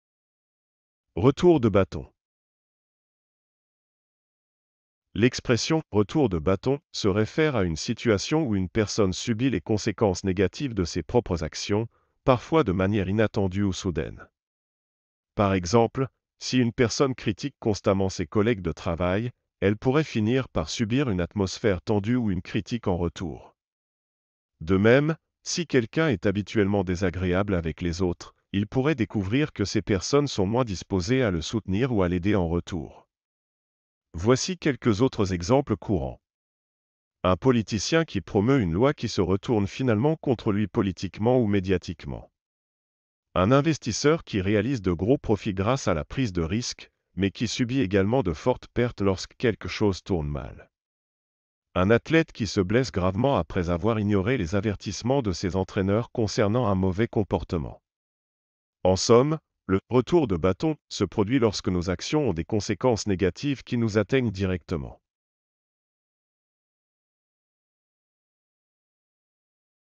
Retour de bâton
L'expression "retour de bâton" se réfère à une situation où une personne subit les conséquences négatives de ses propres actions, parfois de manière inattendue ou soudaine.
Par exemple, si une personne critique constamment ses collègues de travail, elle pourrait finir par subir une atmosphère tendue ou une critique en retour. De même, si quelqu'un est habituellement désagréable avec les autres, il pourrait découvrir que ces personnes sont moins disposées à le soutenir ou à l'aider en retour.
Voici quelques autres exemples courants :
- Un politicien qui promeut une loi qui se retourne finalement contre lui politiquement ou médiatiquement.
- Un investisseur qui réalise de gros profits grâce à la prise de risque, mais qui subit également de fortes pertes lorsque quelque chose tourne mal.
- Un athlète qui se blesse gravement après avoir ignoré les avertissements de ses entraîneurs concernant un mauvais comportement.
En somme, le "retour de bâton" se produit lorsque nos actions ont des conséquences négatives qui nous atteignent directement.